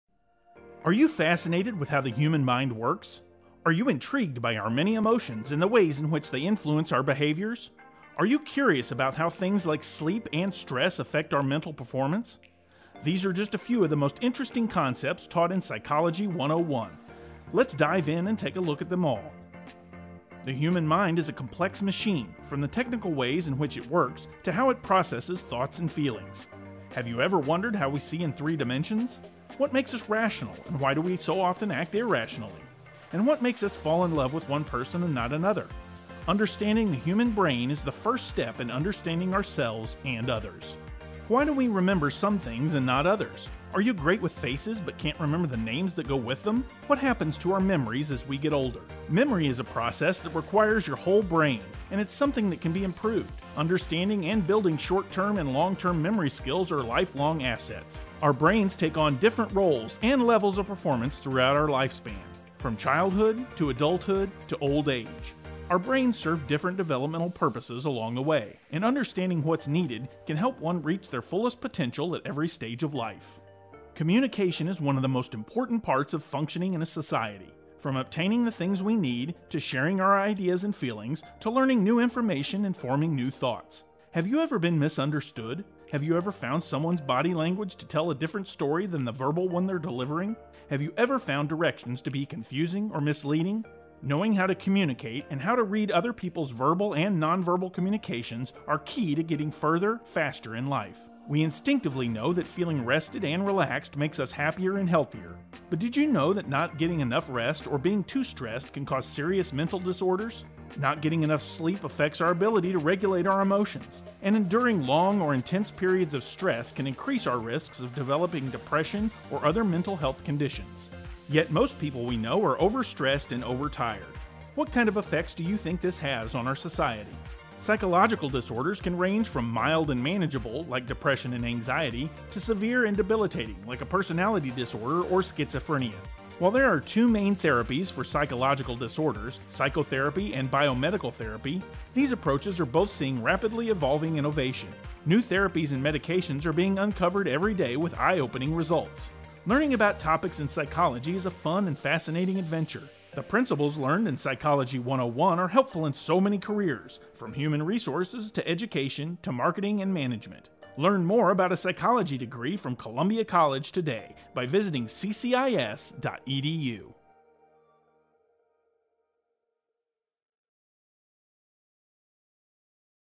6 Interesting Concepts Taught in Psych 101 - Columbia College
Introduction to the field of psychology and the major subareas including the biological basis of behavior, sensation, perception, learning, memory, motivation, emotion, personality, stress, as well as abnormal, developmental and social psychology.